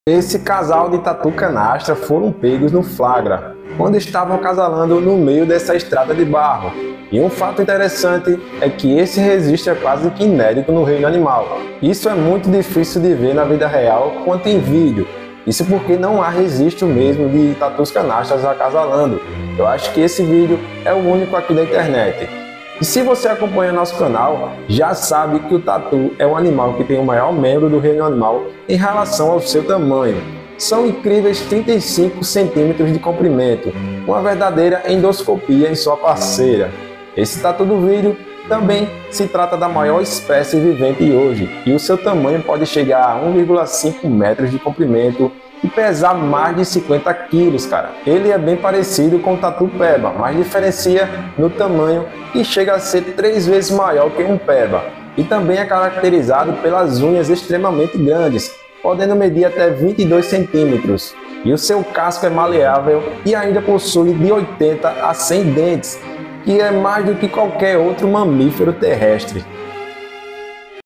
casal De TATU CANASTRA pegos no Flagra! 😱😱😱
These couple of CANASTRA armadillos were caught in the act, when they were mating in the middle of this dirt road. and an interesting fact is that this record is almost unprecedented in the animal kingdom. this is almost as hard to see in real life as it is on video. if you follow our channel, you already know that the armadillo is the animal that has the largest member of the animal kingdom, in relation to its size. they are amazing 35cm long. A real endoscopy on your partner. This armadillo in the video is also the largest living species today. Its size can reach 1.5 m in length, and weigh more than 50 kilos. It is very similar to the armadillo peba, but differs in size, which is up to 3 times larger than a peba. and also It is characterized by extremely large nails, which can measure 22cm. Their shell is malleable, and they also have 80 to 100 teeth, which is more than any other land mammal.

Casal CANASTRA de tatu gigante é pego em flagrante ! 😱😱😱

Esse casal de tatus CANASTRA foi pego em flagrante, quando estavam acasalando no meio dessa estrada de terra. e um fato interessante é que esse acontecimento é quase inédito no reino animal. isso é quase tão difícil de ver na vida real quanto em vídeo. se você acompanha nosso canal, já sabe que o tatu é o animal que possui um dos maiores membros reprodutores do reino animal, em relação ao seu tamanho, medindo incríveis 35 cm de comprimento, Uma verdadeira endoscopia em seu parceiro.
Este tatu do vídeo também é a maior espécie viva da atualidade. Seu tamanho pode chegar a 1,50 m de comprimento e pesar mais de 50 quilos. É muito parecido com o tatu peba, mas difere no tamanho, que chega a ser 3 vezes maior que o peba. e também É caracterizado por unhas extremamente grandes, que o auxiliam na escavação de tuneis gigantescos embaixo da terra, essas garras poderosas podem medir 22cm. Sua concha é maleável e eles também possuem de 80 a 100 dentes, o que é mais do que qualquer outro mamífero terrestre.